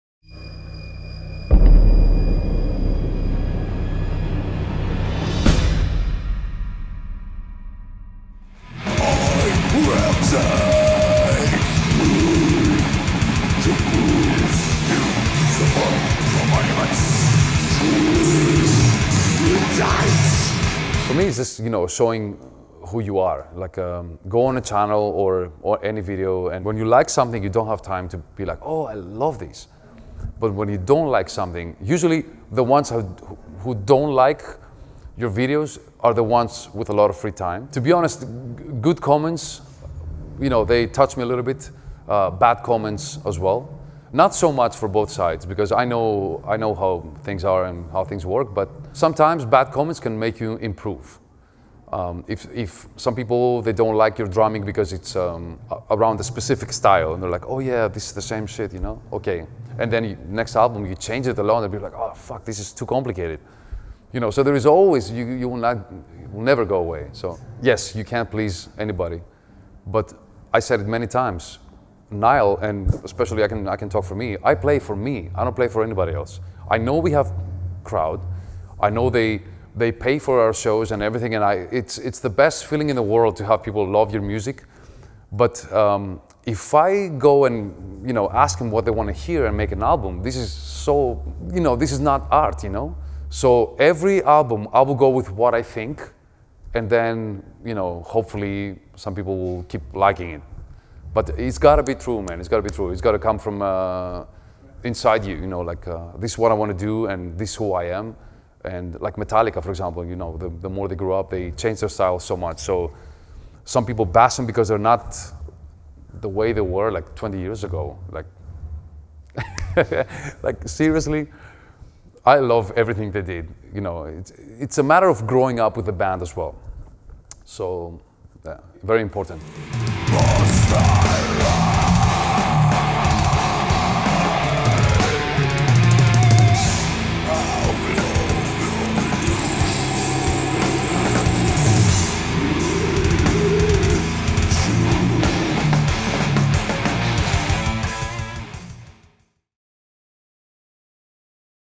George Kollias on Internet comments - drumtalk [single strokes 15]
The internet is both a blessing and a destructive place -- how does positive and negative reactions and comments affect you on a personal level and as a musician? Does it affect your playing? George Kollias has a very healthy take on this issue.